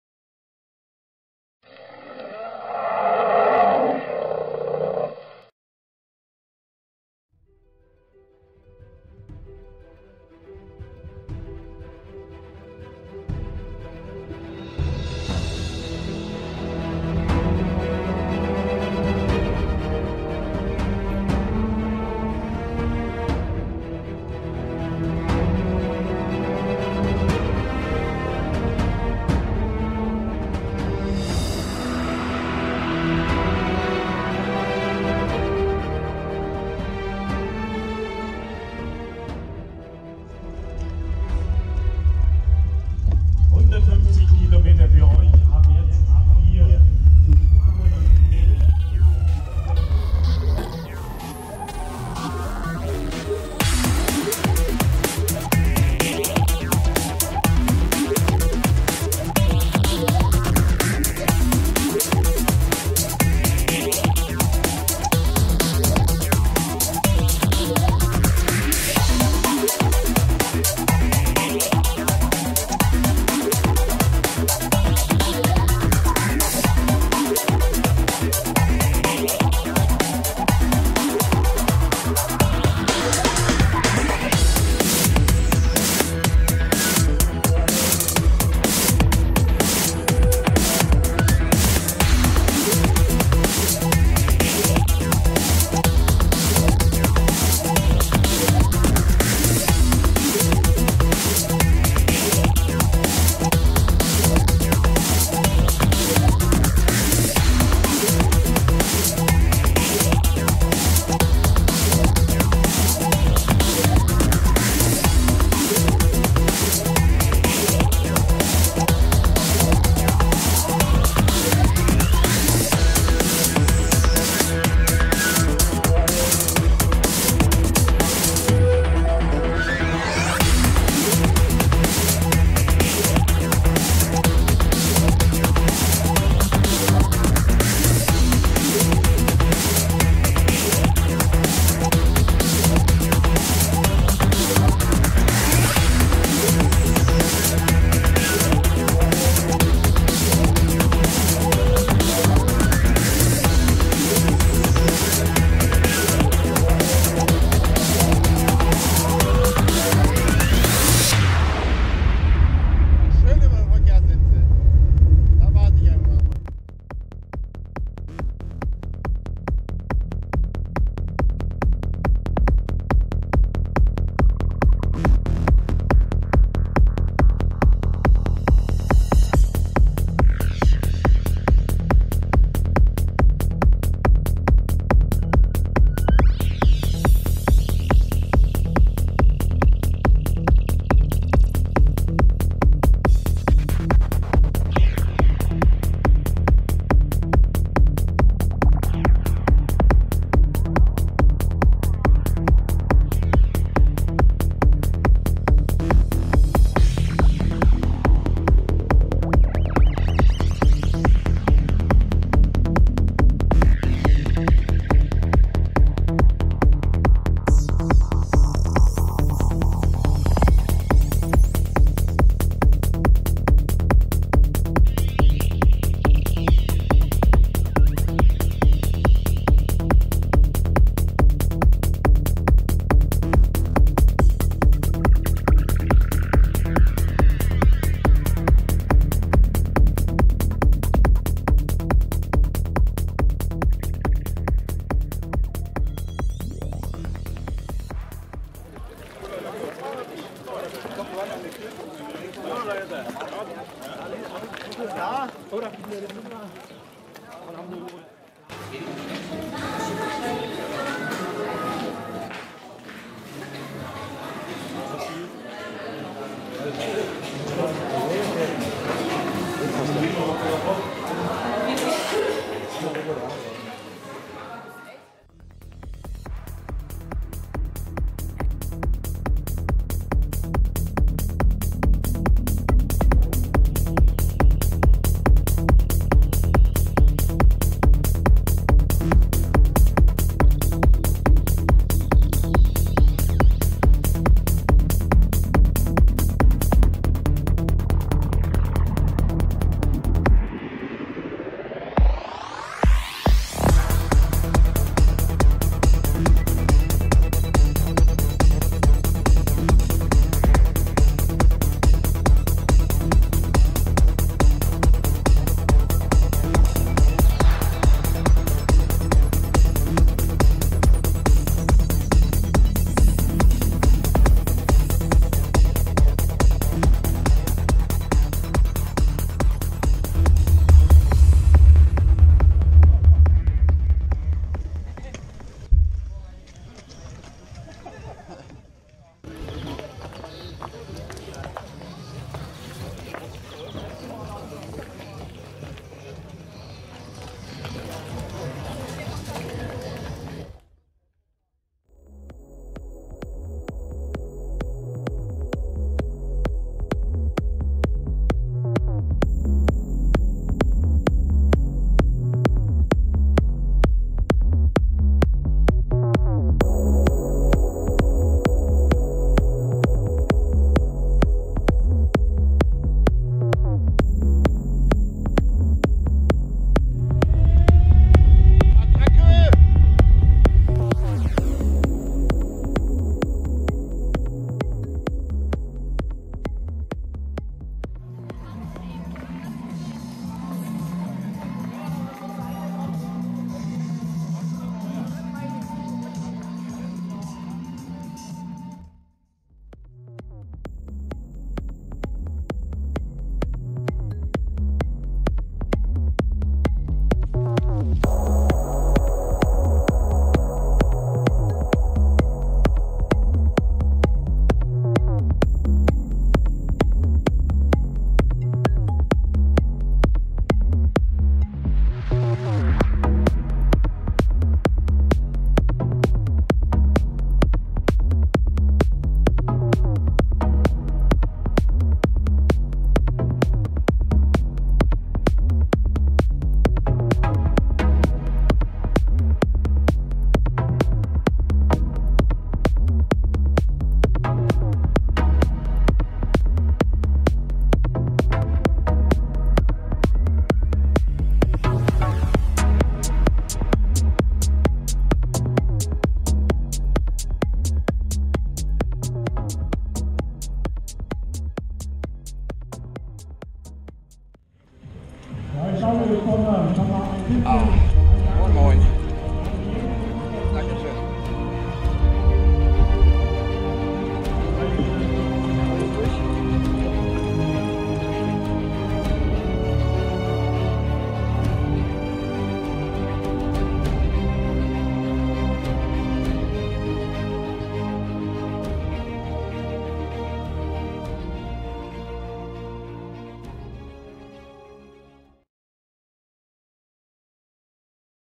150 km Spreewald Marathon 2016
Heute ging es in den Spreewald zu meiner ersten richtigen RTF-Veranstaltung. Mit der 150 km Strecke wollte ich meinen Tages-KM-Rekord verbessern. Bei schönem Wetter gestartet, hat ab km 85 Regen eingesetzt, der bis fast zum Schluss anhielt. Schön ist anders. Zum Schluss waren es 153 km in einer Fahrzeit von 5:21 Stunden.